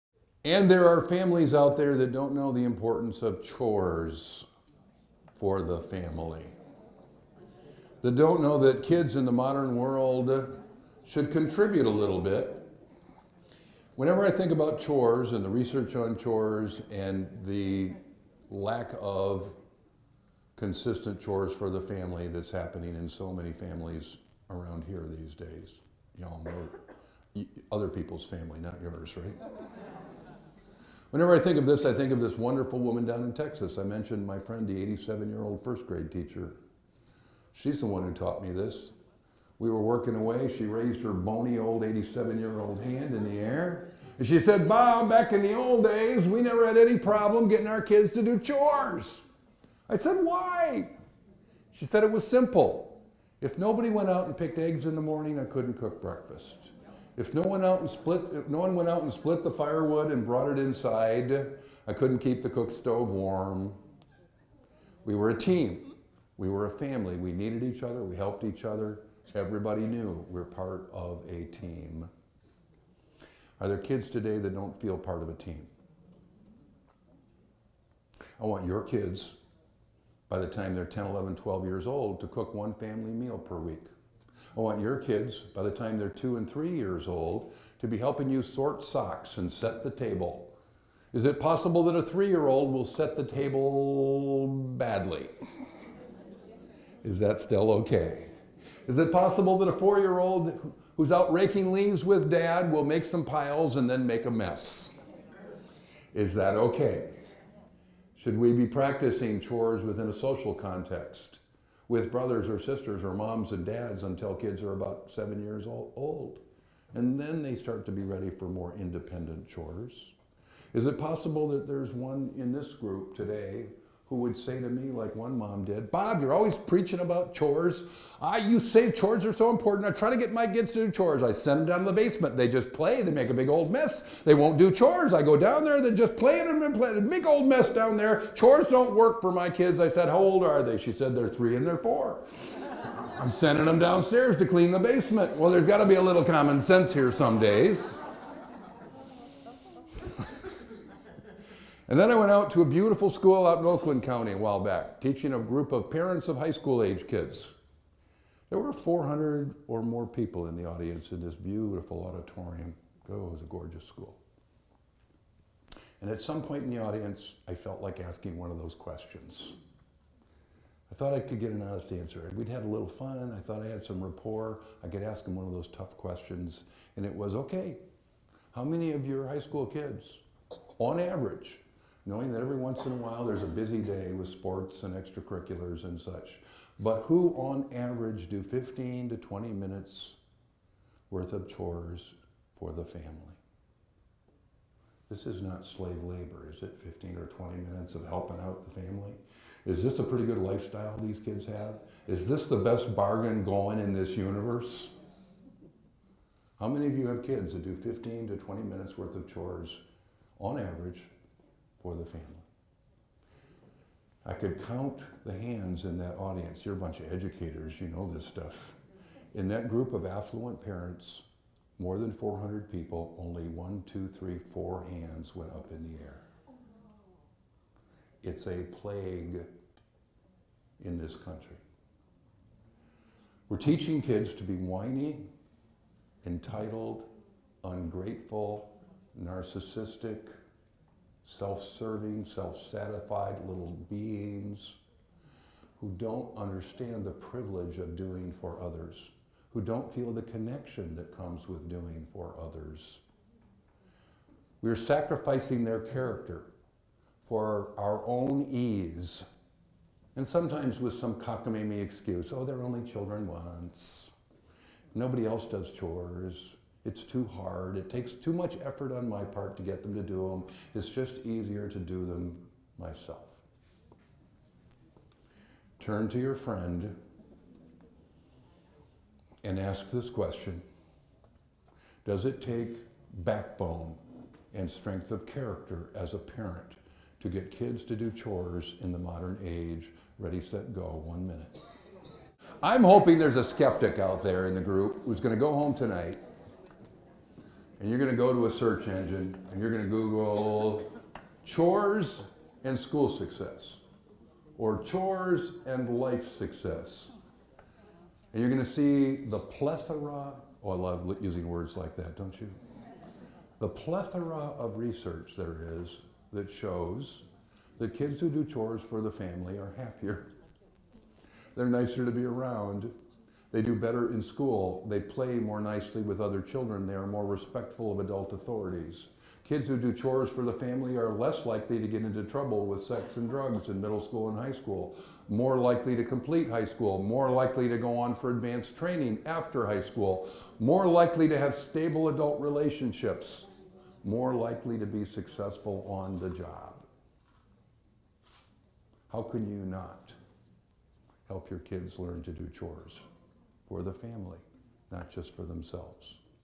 Chores in the Modern Age
How chores help kids become successful, happy human beings.